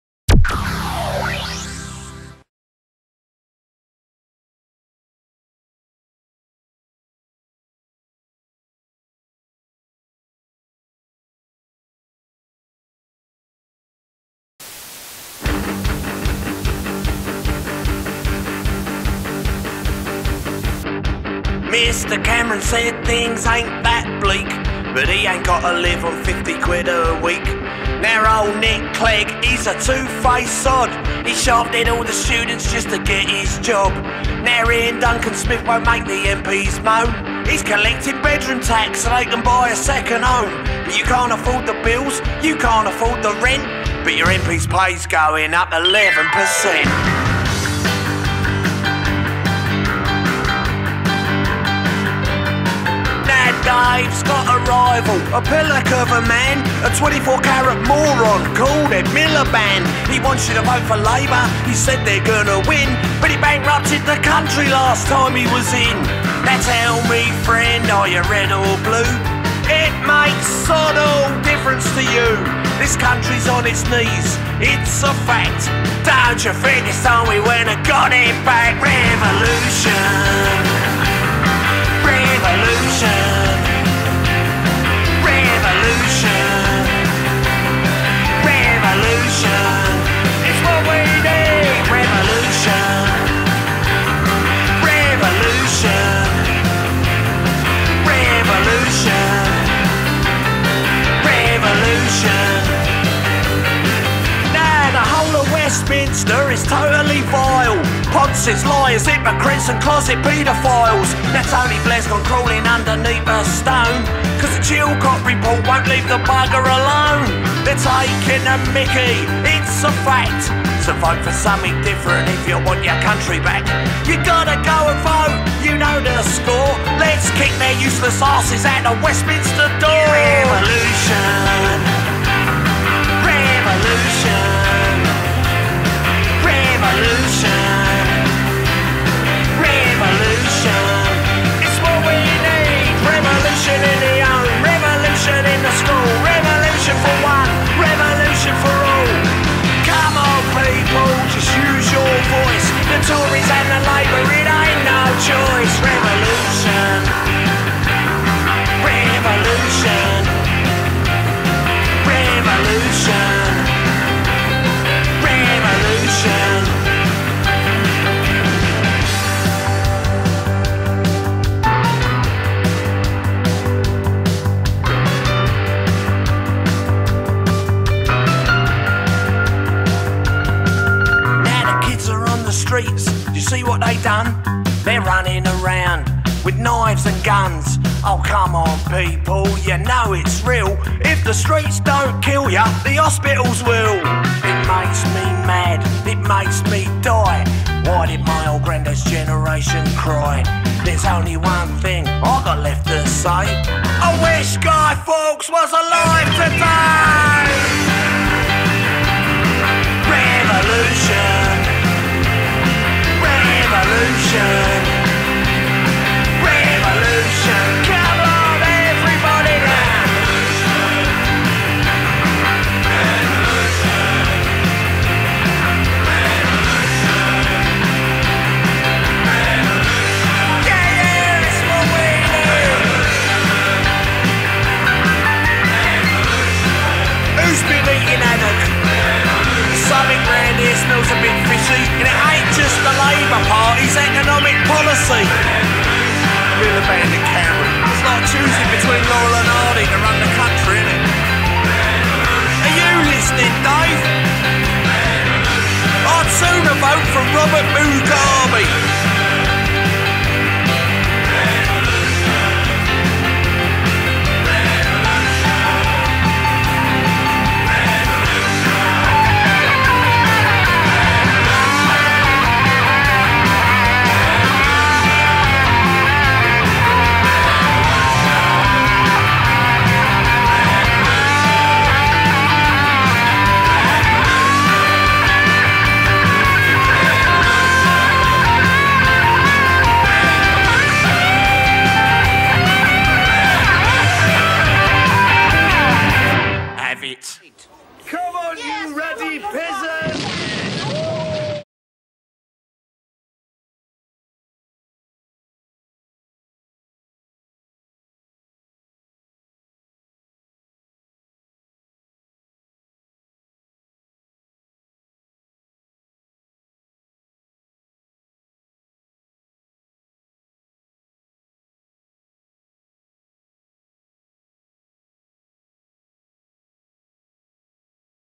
Citizen Steve - REVOLUTION 2015
Citizen Steve's 2015 General Election broadcast.